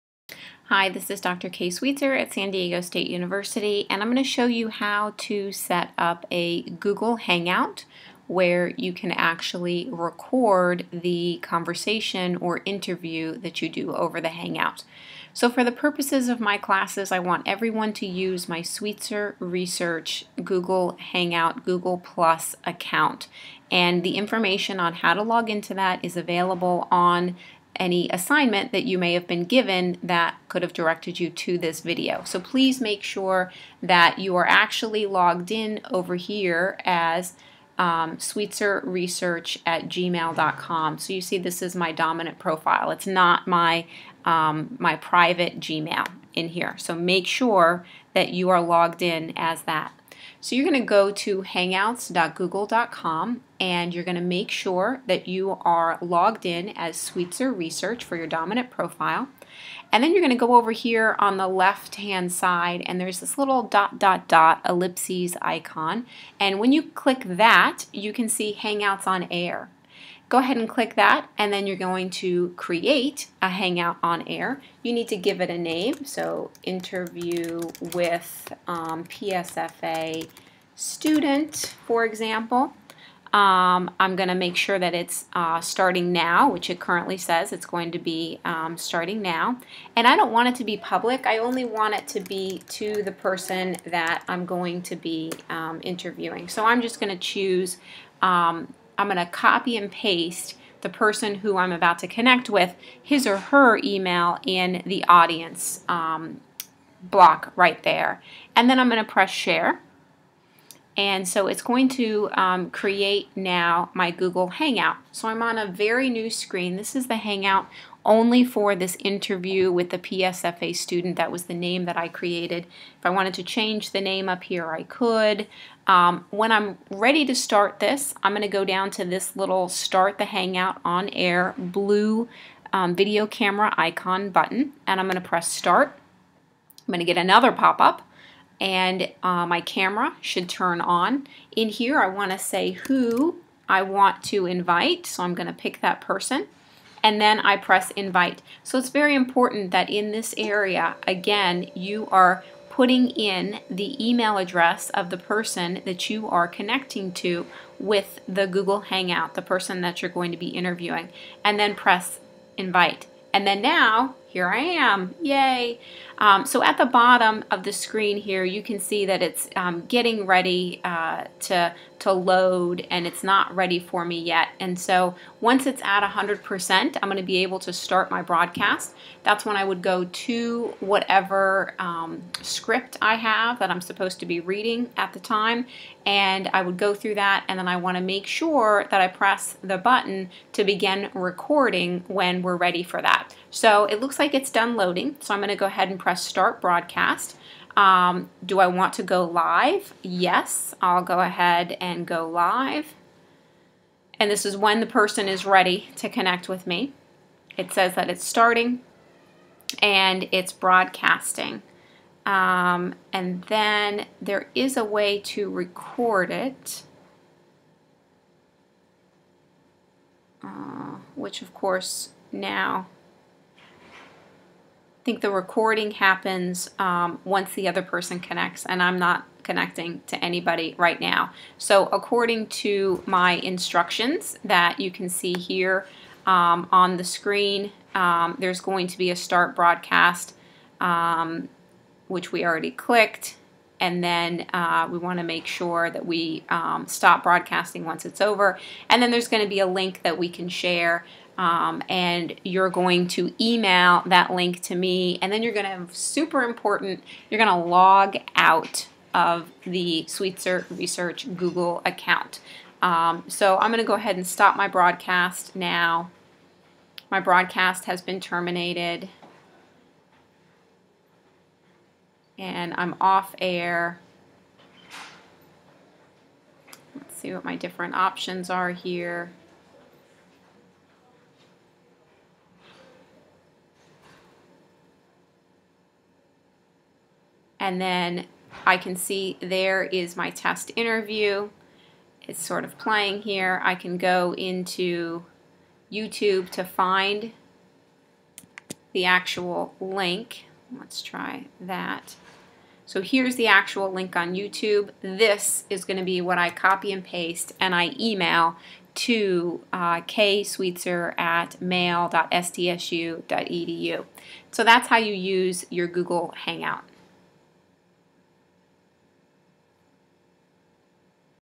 How to Set Up a Google Hangout
This is a screen grab video to show you how to set up a Google Hangout, which would be useful if you're conducting an interview.